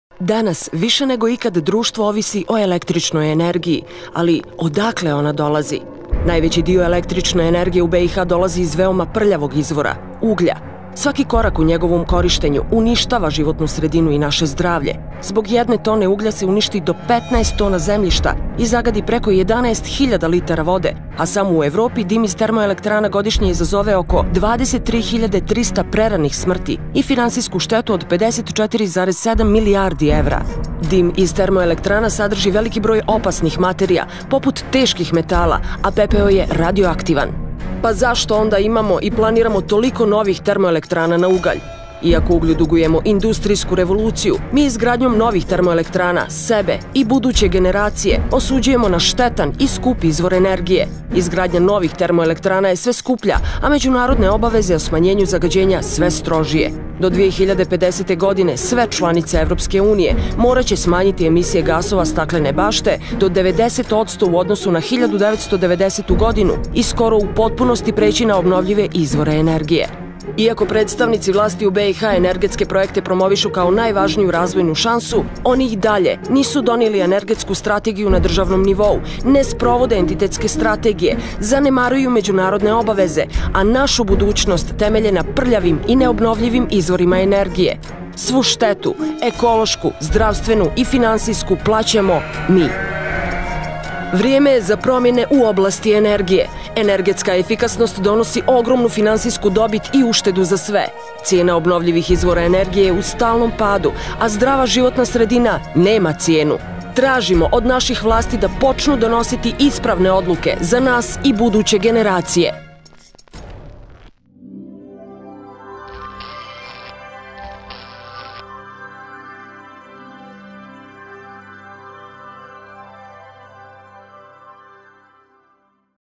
Stop prljavoj energiji - jer budućnost je obnovljiva
Kampanju Stop prljavoj energiji - jer budućnost je obnovljiva realizuju Centar za životnu sredinu iz Banja Luke i Centar za ekologiju i energiju iz Tuzle. Kampanja je nastala kao odgovor na neodgovorno i neodrživo upravljanje i planiranje energetskog sistema Bosne i Hercegovine sa velikim udjelom izuzetno opasnog i prljavog uglja. U ovoj kratkoj animaciji ćete moći dobiti više informacija o opasnostima ovakvog pristupa proizvodnji električne energije ali i o alternativnim riješenjima.